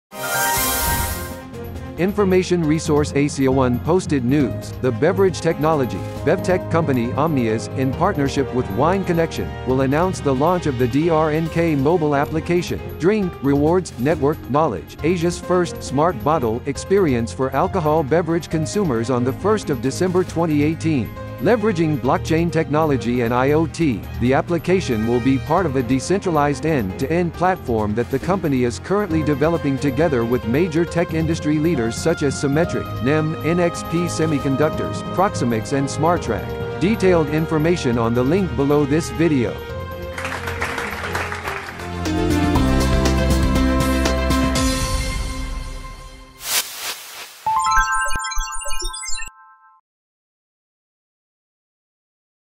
KCN Buying alcohol through a blockchain
Information resource asiaone posted news: The beverage technology (BevTech) company Omniaz, in partnership with Wine Connection, will announce the launch of the DRNK mobile application (Drink, Rewards, Network, Knowledge), Asia's first 'Smart Bottle' experience for alcohol beverage consumers on 1 December 2018. Leveraging Blockchain technology and IoT, the application will be part of a decentralized end-to-end platform that the company is currently developing together with major tech industry leaders such as CymMetrik, NEM, NXP Semiconductors, ProximaX and Smartrac. Detailed information on the link below this video.